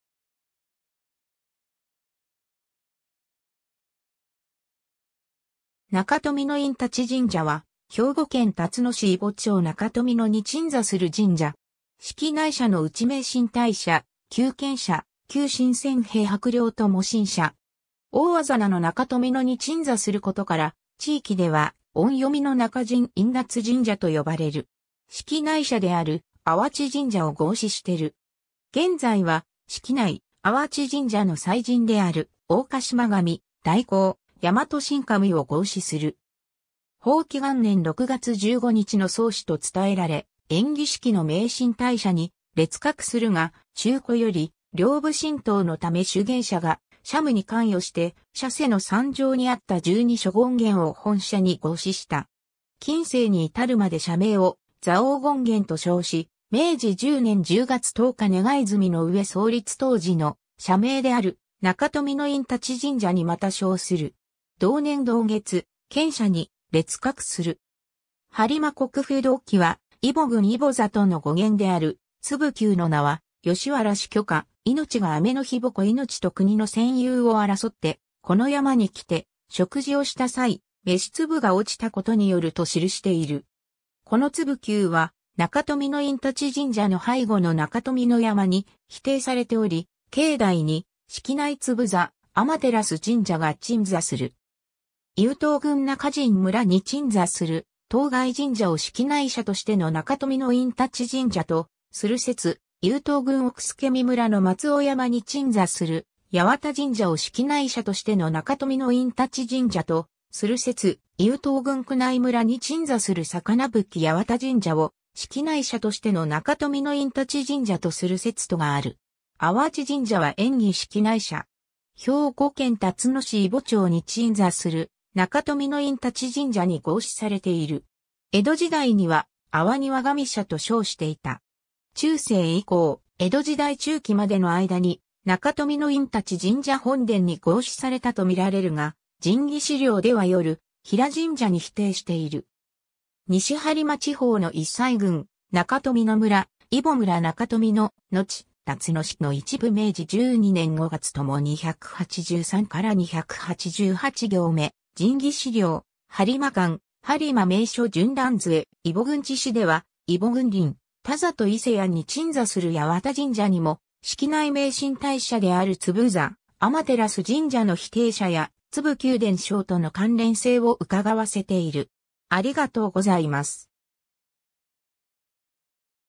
中臣印達神社
中臣印達神社（なかとみ いたてじんじゃ）は、兵庫県たつの市揖保町中臣に鎮座する神社。
式内社のうち名神大社、旧県社、旧神饌幣帛料供進社。
大字名の中臣（なかじん）に鎮座することから、地域では音読みのナカジン-インダツ神社と呼ばれる。
式内社である阿波遅神社を合祀してる。
 現在は、式内阿波遅神社の祭神である大鹿嶋神・大香山戸臣神を合祀する。
 宝亀元年6月15日の創祀と伝えられ、延喜式の名神大社に列格するが、中古より両部神道のため修験者が社務に関与して社背の山上にあった十二所権現を本社に合祀した。
近世に至るまで社名を蔵王権現と称し、明治10年10月10日願済の上創立当時の社名である中臣印達神社に復称する。
同年同月、県社に列格する。
 播磨国風土記は、揖保郡揖保里の語源である「粒丘」の名は、『葦原志挙乎命が天日槍命と国の占有を争ってこの山に来て食事をした際、飯粒が落ちたことによる』と記している。
この粒丘は中臣印達神社の背後の中臣山に比定されており、境内に式内粒坐天照神社が鎮座する。
 揖東郡中陳村（たつの市揖保町中臣）に鎮座する当該神社を式内社としての中臣印達神社とする説、揖東郡奥佐見村（姫路市林田町八幡）の松尾山に鎮座する八幡神社を式内社としての中臣印達神社とする説、揖東郡宮内村（姫路市網干区宮内）に鎮座する魚吹八幡神社を式内社としての中臣印達神社とする説とがある。
 阿波遅神社（あわちじんじゃ）は延喜式内社。
兵庫県たつの市揖保町に鎮座する中臣印達神社に合祀されている。
江戸時代には阿波庭神社と称していた。
 中世以降、江戸時代中期までの間に中臣印達神社本殿に合祀されたとみられるが、『神祇志料』では夜比良神社に比定している。
 ^ 西播磨地方の揖西郡中臣村（中陳村）、揖保村中臣、のち龍野市の一部 ^ 明治12年5月とも ^ 283-288行目 ^ 「神祇志料」「播磨鑑」「播磨名所巡覧図会」「揖保郡地誌」では揖保郡林田里伊勢野（姫路市林田町上伊勢）に鎮座する八幡神社にも、式内名神大社である粒坐天照神社の比定社や粒丘伝承との関連性を窺わせている。